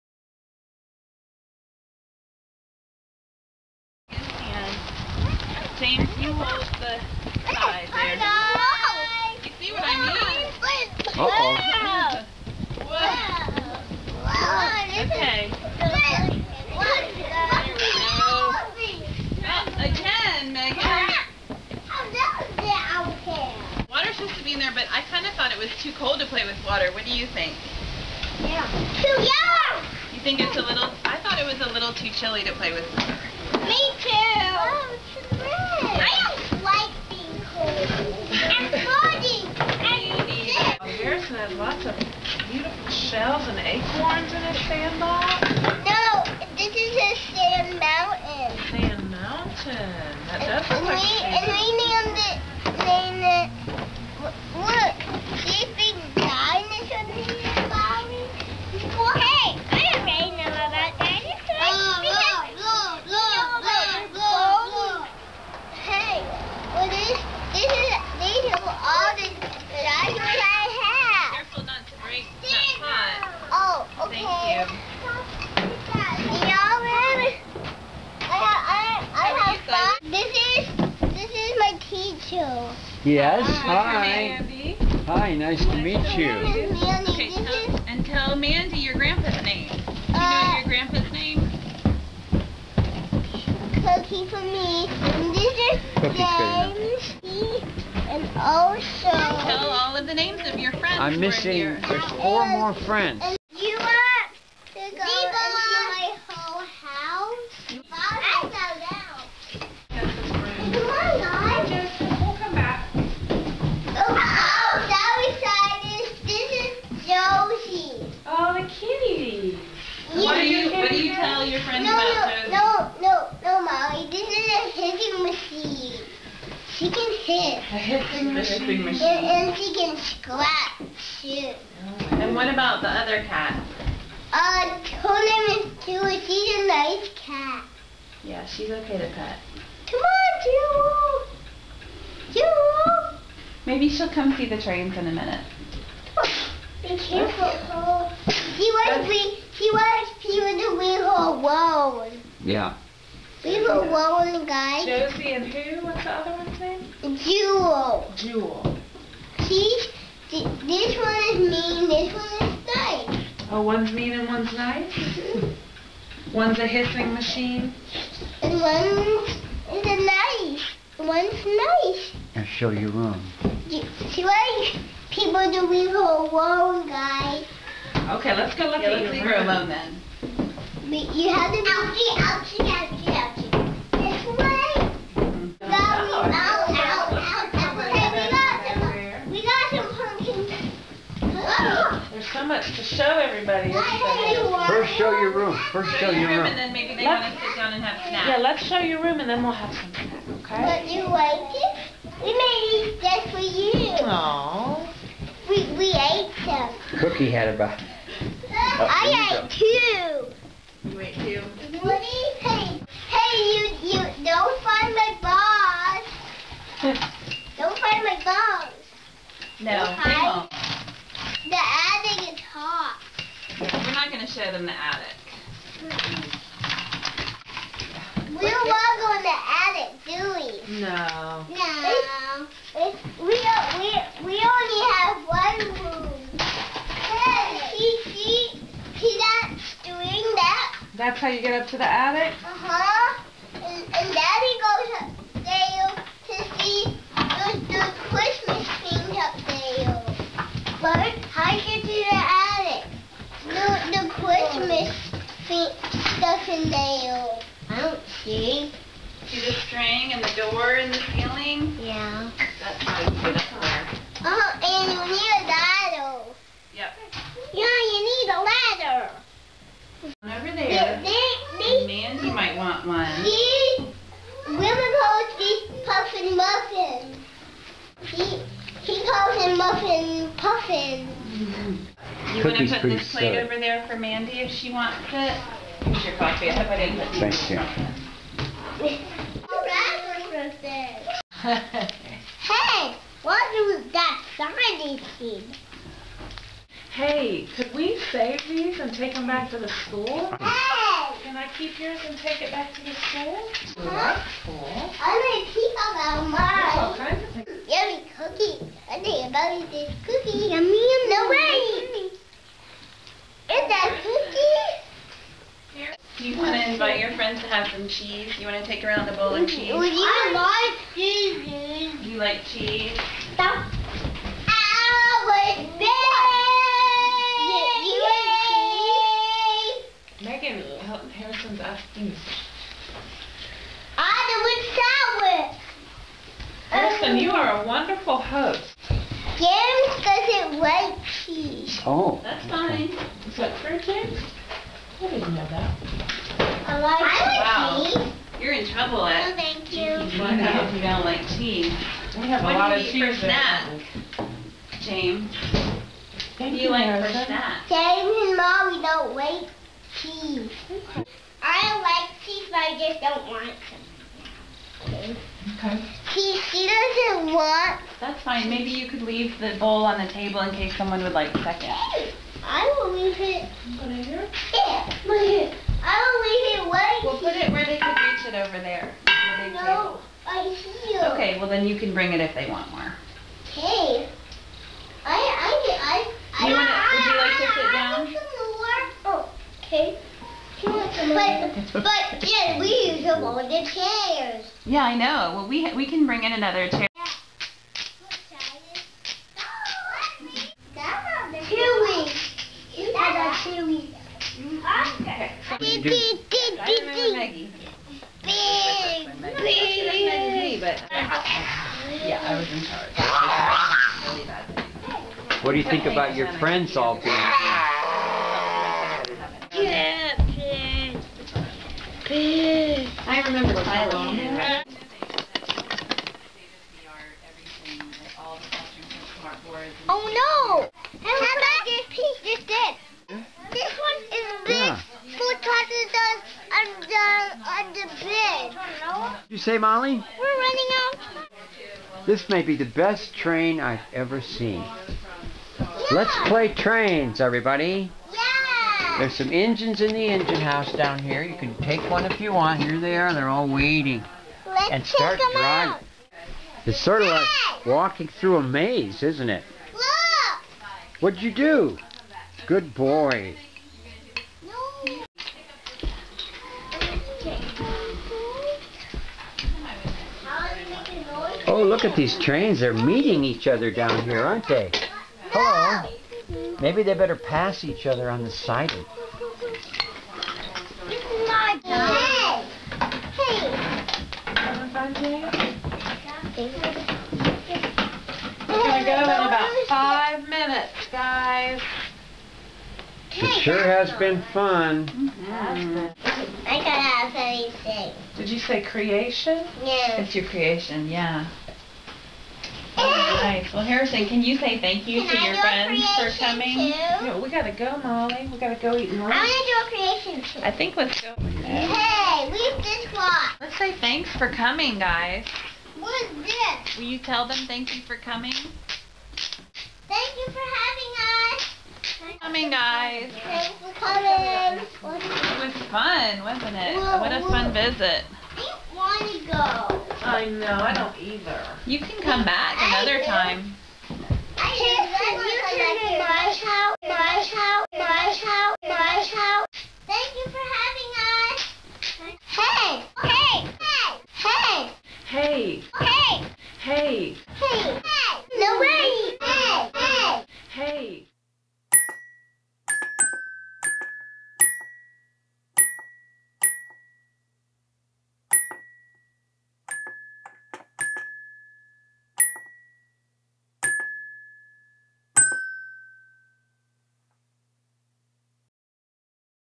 Friends come to visit
Preschoolers from Grant Park Cooperative Preschool come to visit their friend at home.  There are introductions, a tour, a snack, and some play time.  The host does a great job explaining everything, especially the difference between the good cat and the 'hissing machine' cat.